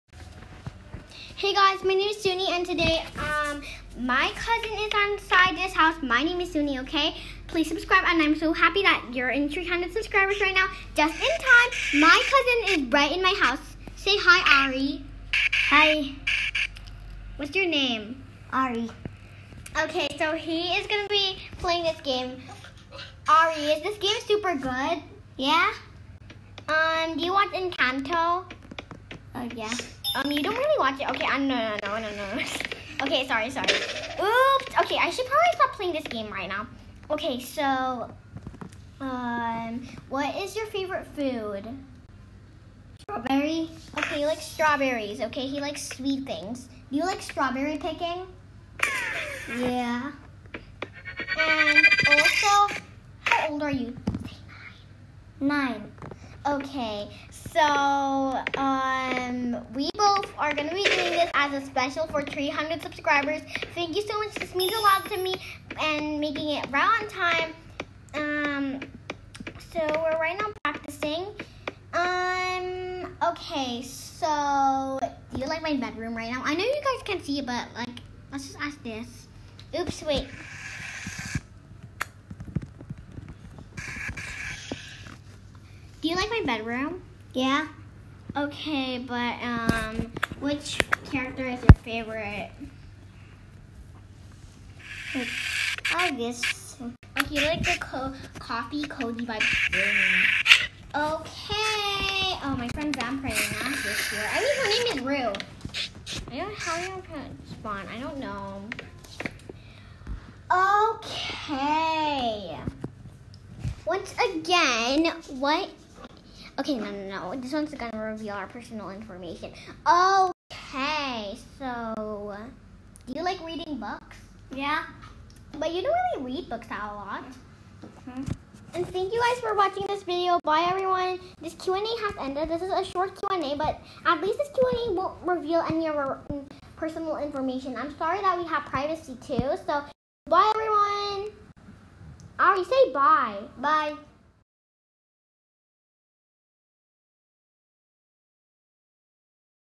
interview with my brother ari! | late 300 subscribers special
•._.••´¯``•.¸¸.•` хuní ís tчpíng `•.¸¸.•´´¯`••._.•

welcome to moon kitty paradise! 🌙✨🌌

i'm sorry, i made this video on tuesday but i uploaded it today!
i hope you enjoy the video, i also revealed my brother's voice!

•´¯`•. hσw much tímє díd ít tαkє? .•´¯`•

this took me 2 minutes!

•´¯`•. crєdíts .•´¯`•

none

•´¯`•. shσutσuts .•´¯`•

sorry, none today!